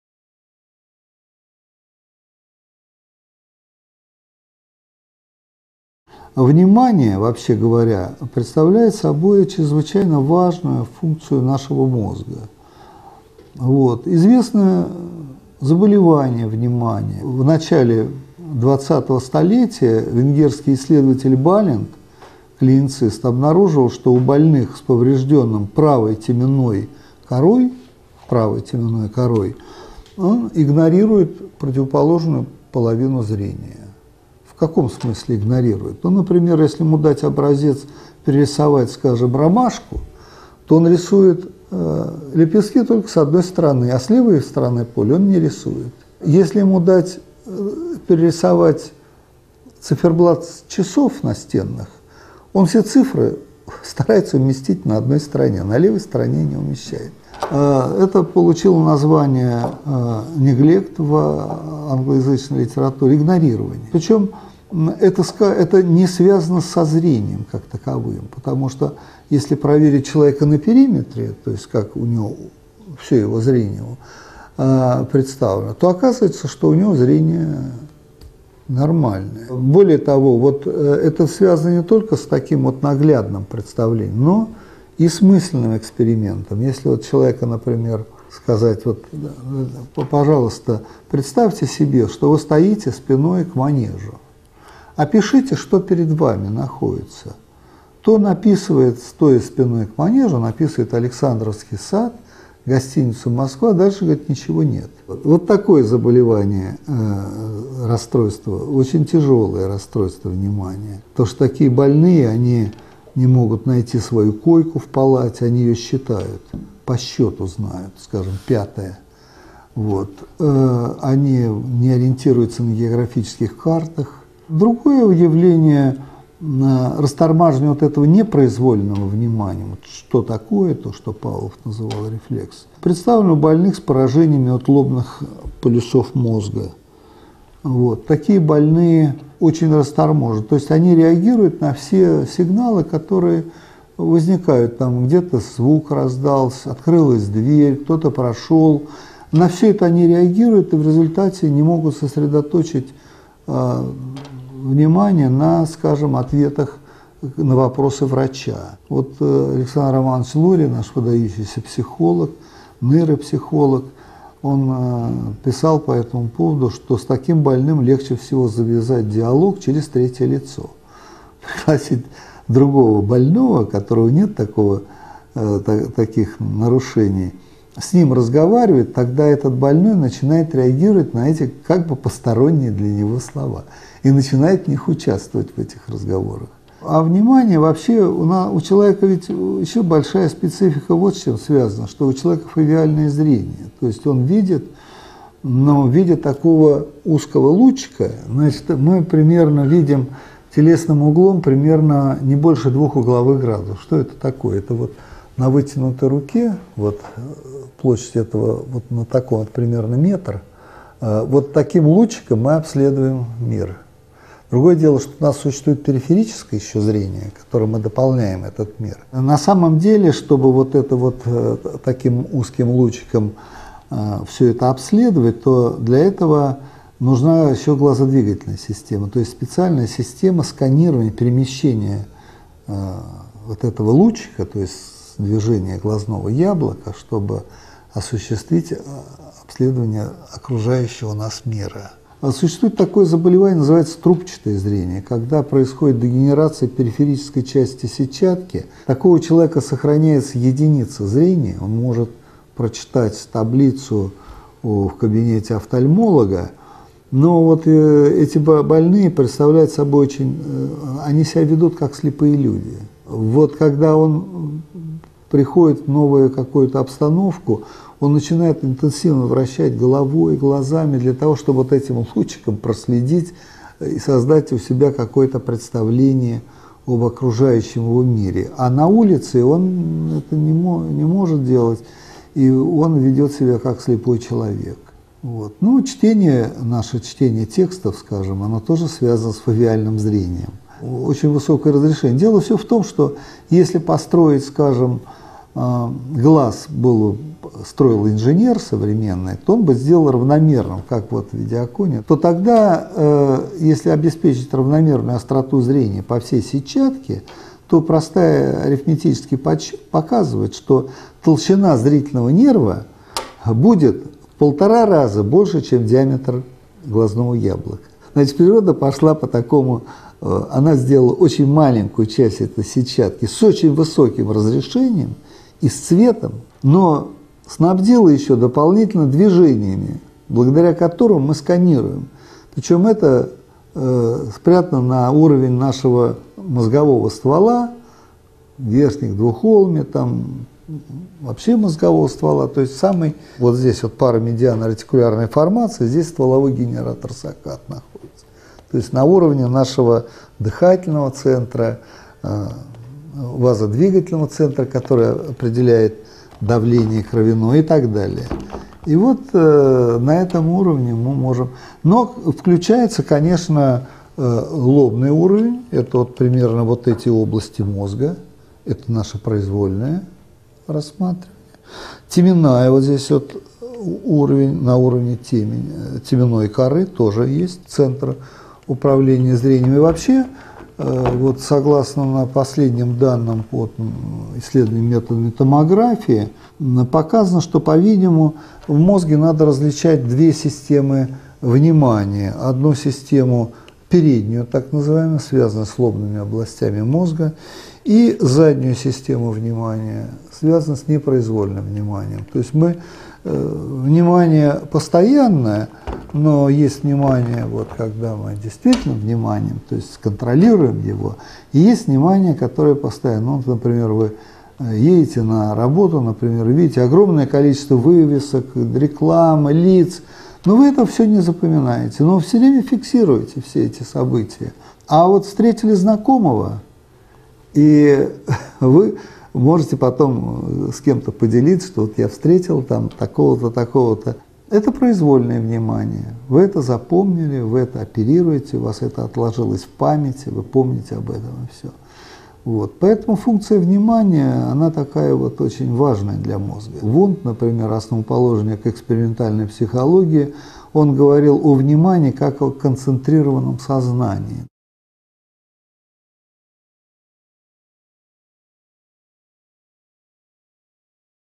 Мозг человека: Внимание и зрение - Шульговский Валерий
Шульговский Валерий Виктoрович - доктор биологических наук, профессор, заведующий кафедрой высшей нервной деятельности биологического факультета МГУ, руководитель лаборатории системного анализа деятельности мозга.